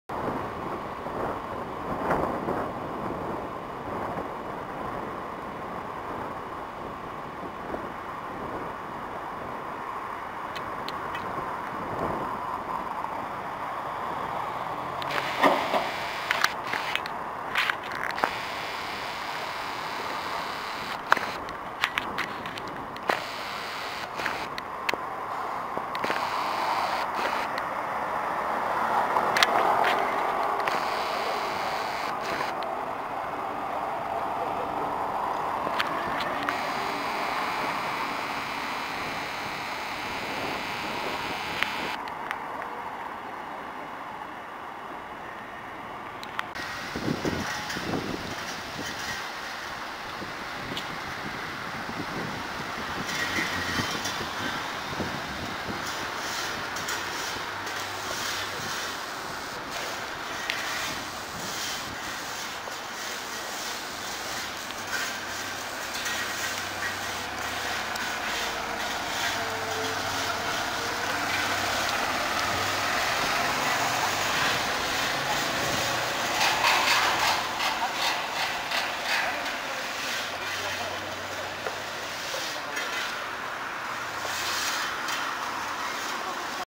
2021.02.16 日野ブルーリボンハイブリッド 2SG-HL2ASBP 近鉄バス 0751 (鳥飼) 大阪200か44-09 「24 阪大病院前」「22 JR･阪急茨木」 阪大医学部前バス停付近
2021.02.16 阪大医学部前バス停付近
日野ブルーリボンハイブリッド 2SG-HL2ASBP ノンステWB6.0m
大阪200か44-09 (近鉄バス 鳥飼営業所所属 0751)
ラッピング
(後面) エビスホーム
「24 阪大病院前」行き
「22 JR･阪急茨木」行き